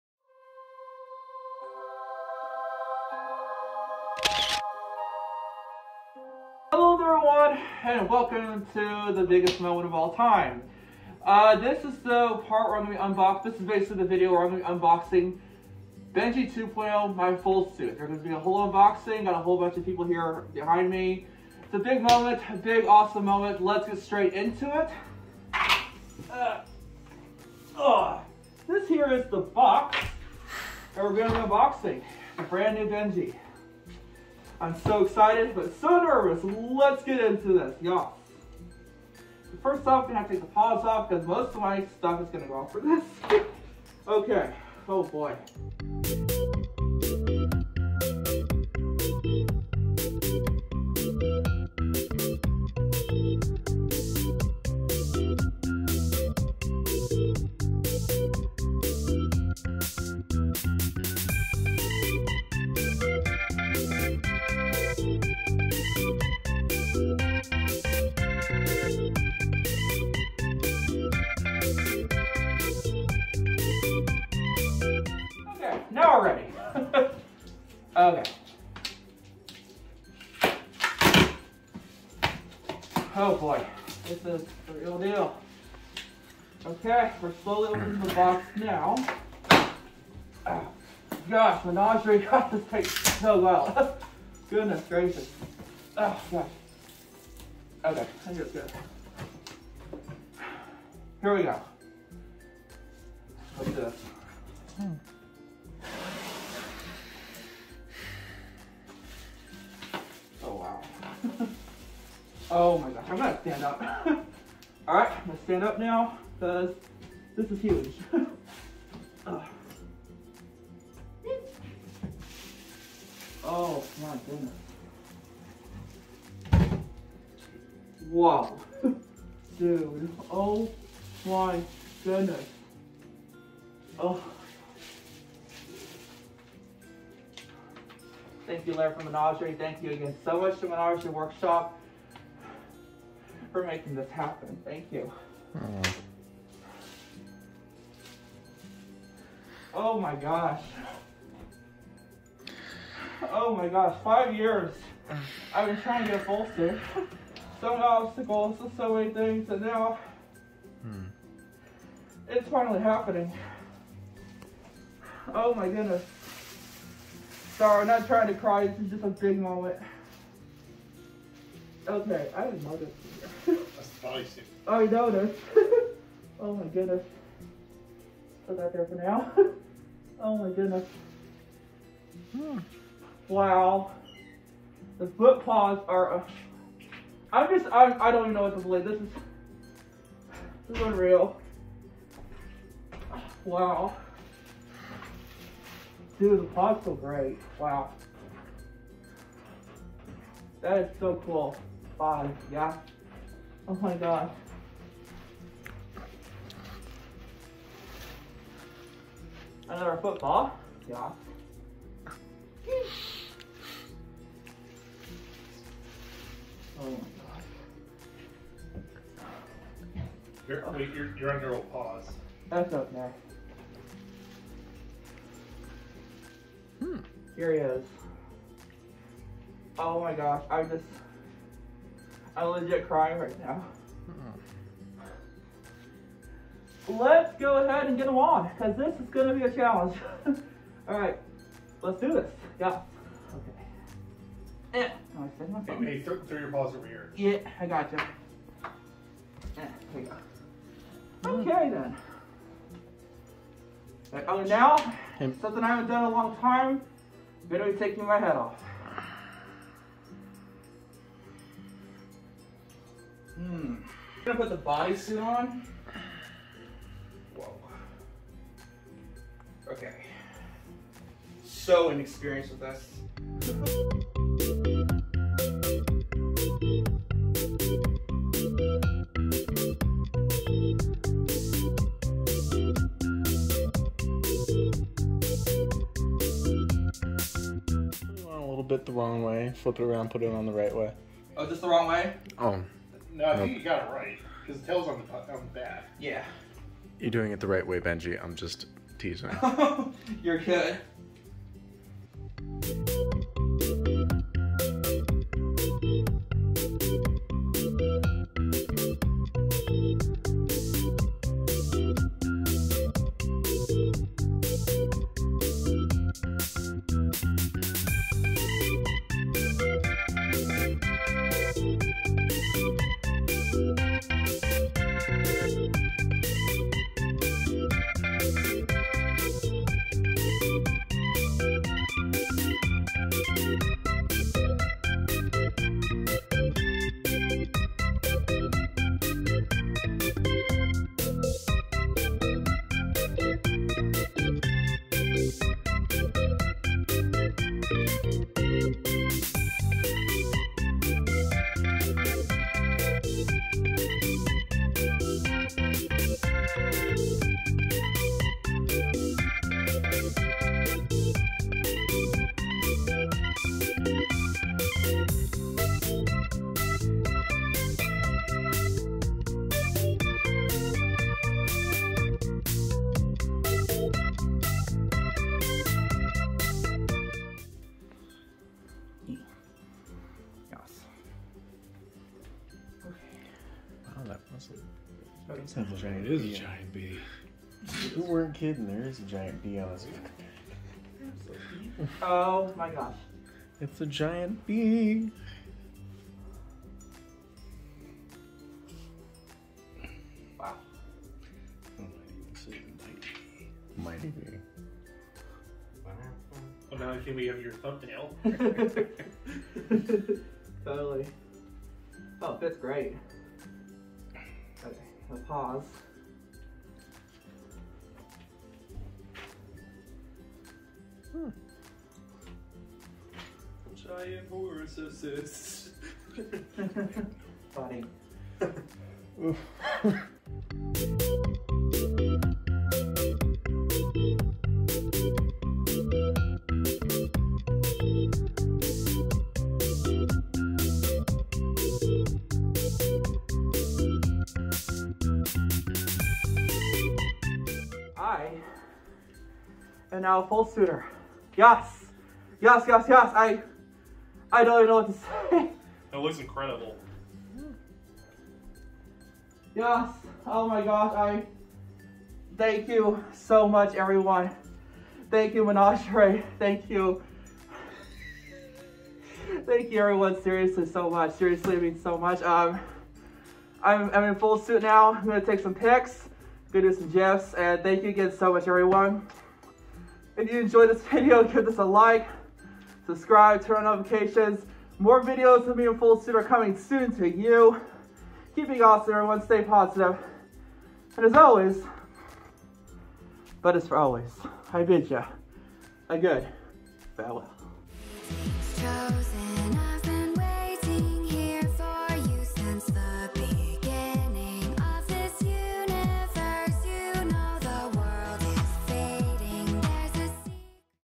BENJI 2.0 FULLSUIT UNBOXING!!!
THIS IS THE BIG MOMEMT!!!

IN THIS VIDEO I WILL BE UNBOXING MY FULLSUIT BENJI 2.0!!!

I HOPE YOU ALL ENJOY YAAAAAASSSS

Special thanks to my patrons:
KOREY FIREFOX,MAX THE FOX,WHITEHART,FROST SNOWY DUTCHIE,
Moonshine, Russet, Sparks, Shrapnel, Michalli, Edgecoy, Danny,Timber,Gavin,